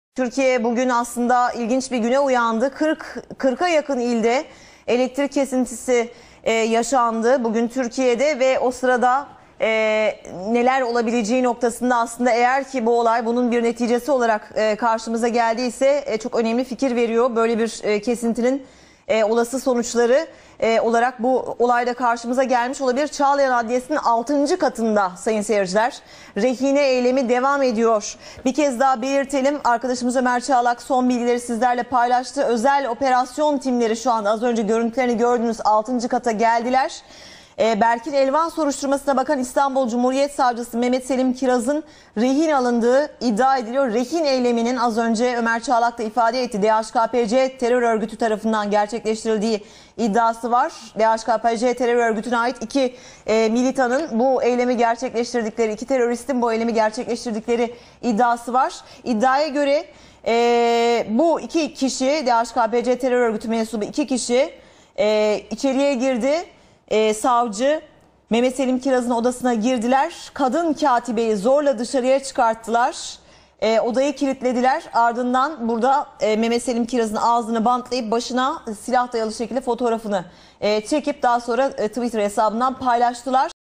Dos terroristas secuestran a un fiscal en los juzgados de Estambul
Miembros del grupo terrorista de extrema izquierda DHKP-C habrían tomado como rehén al fiscal encargado del caso por la muerte de Berkin Elvan, una de las víctimas de las protestas por el Gezi Park.